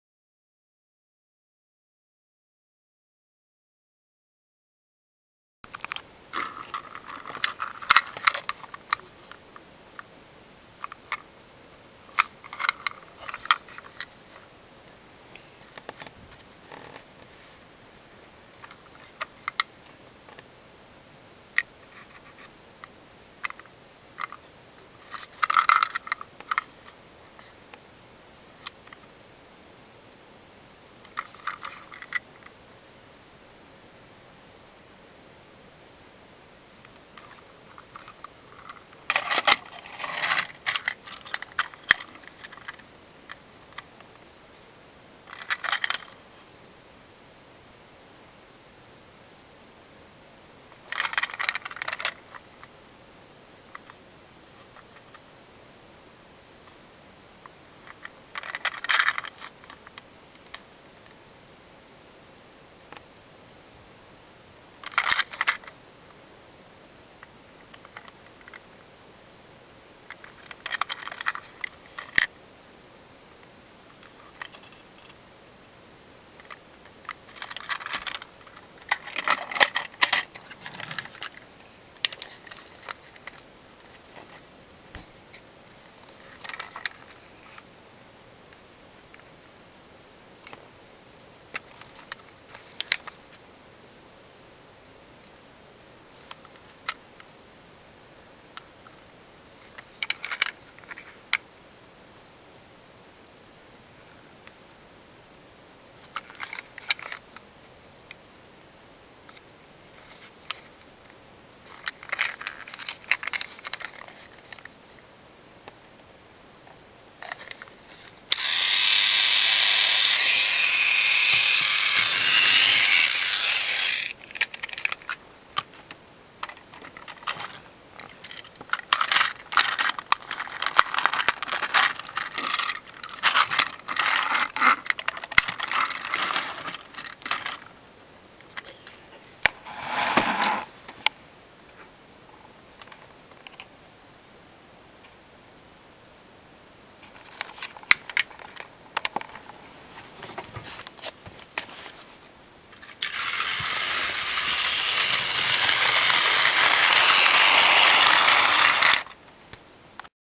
改造プラレール紹介動画 44 「103系1500番台」 バージョン1 0
今回は筑肥線の西唐津～筑前前原間を走る
3両編成の103系1500番台の紹介です。
キュアドリームが説明します。